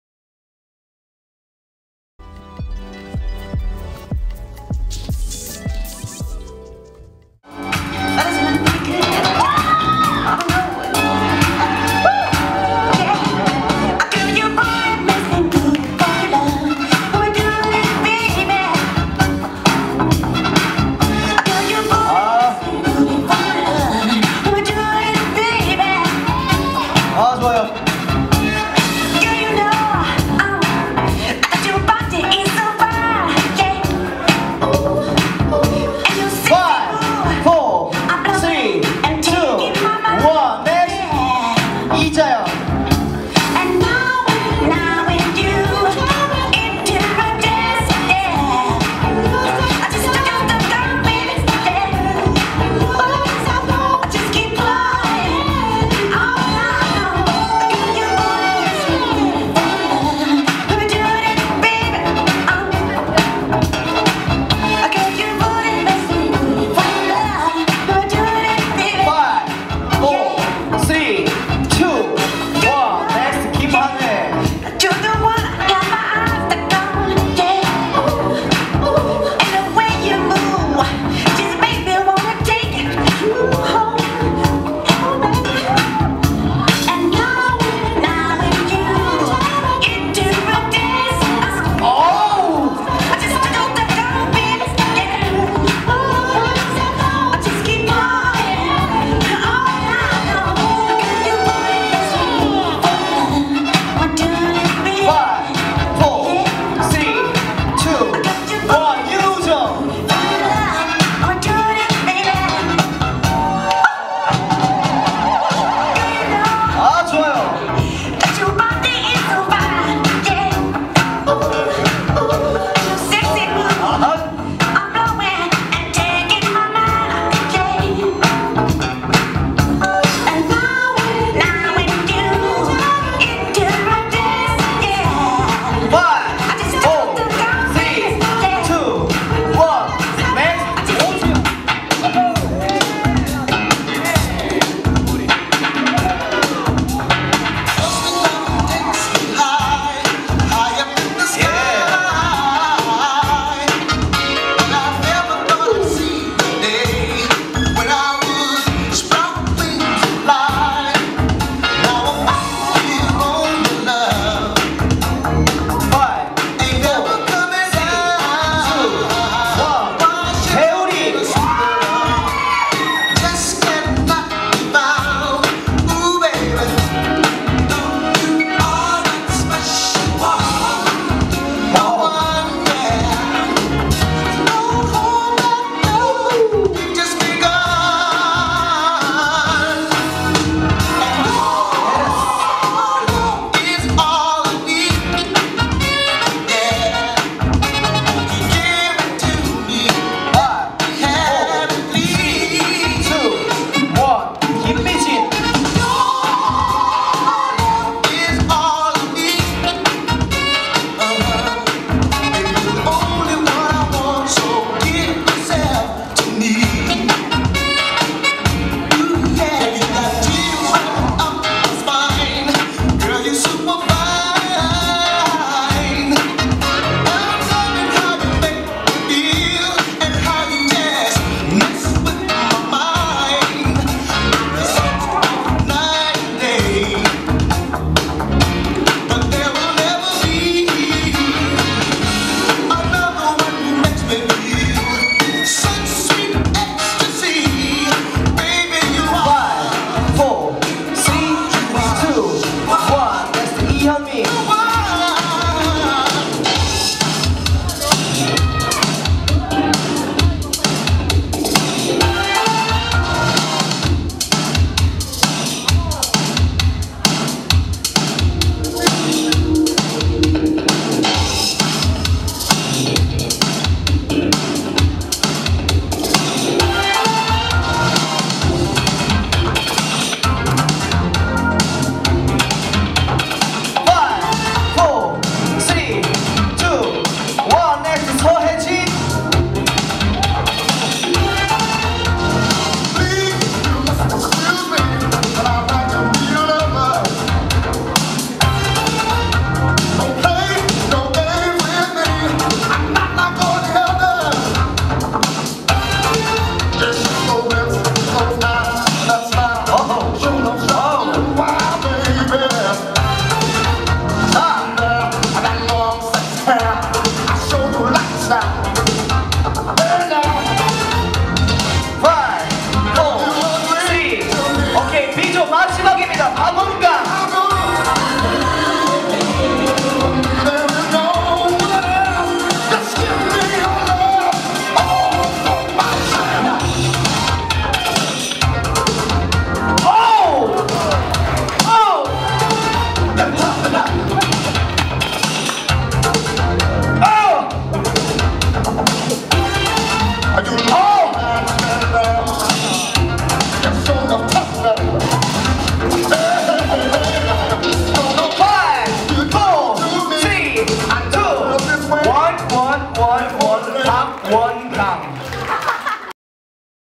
WAACKIN 예선 1차 B조 / KEEP DANCING VOL.11 / Allthatbreak.com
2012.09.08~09
Keep Dancing Vol.11 Prelims @ Ye-Hall, Apgujeong
DAY 1 : Poppin / Lockin / Breakin / Wackin
DAY 2 : Hiphop / House / Krump
PRESENTED BY FLOWMAKER

Waackin Prelims 1 Group B : 박미림, 이자연, 김한희, 유우정, 오주현, 배우리, 김이진, 이현미, 서혜진, 박원강